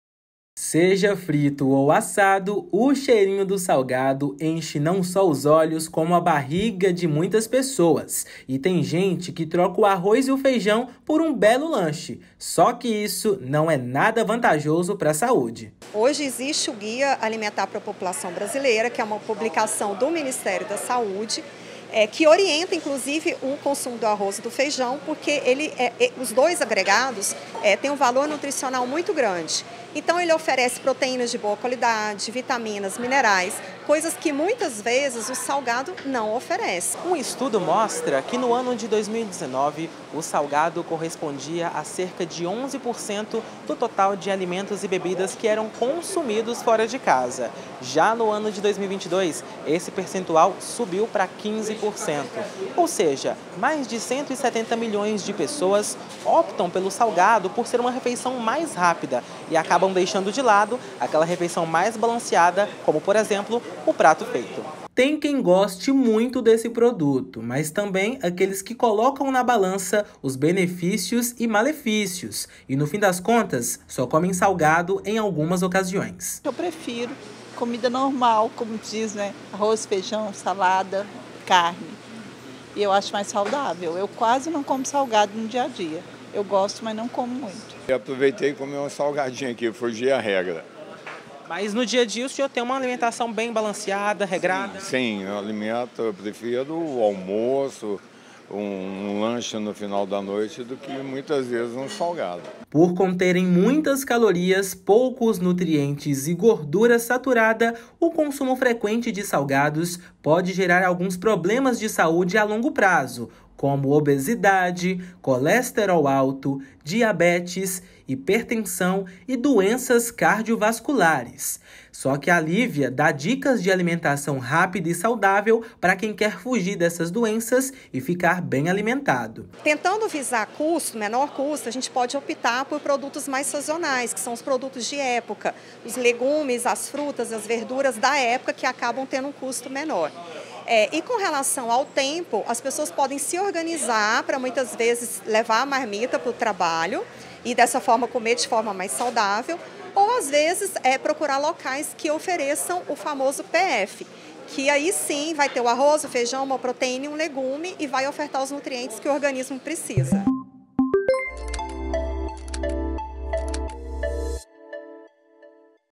POPULAÇÃO SUBSTITUI PRATO DE ARROZ E FEIJÃO POR SALGADOS
Médicos alertam para aparecimento de doenças como diabetes e hipertensão

Rua 260, Qd.72-A, Lt.06, s/n - Setor Leste Universitário, Goiânia - GO, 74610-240